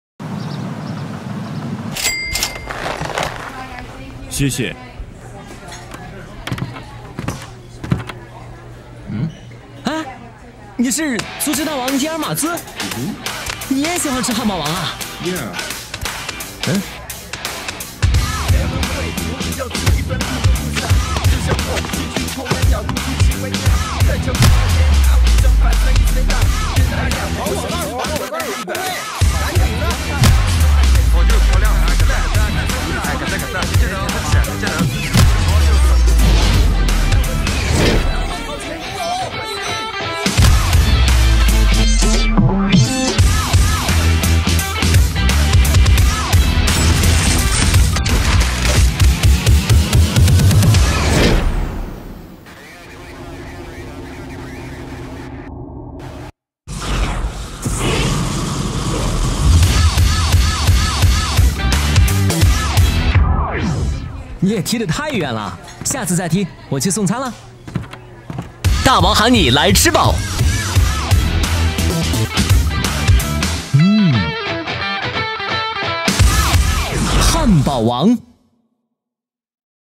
Burak Yılmaz Çin Burger King'i Reklamı 2017
Burak Yılmaz Çin'de Burger King reklam yüzü oldu!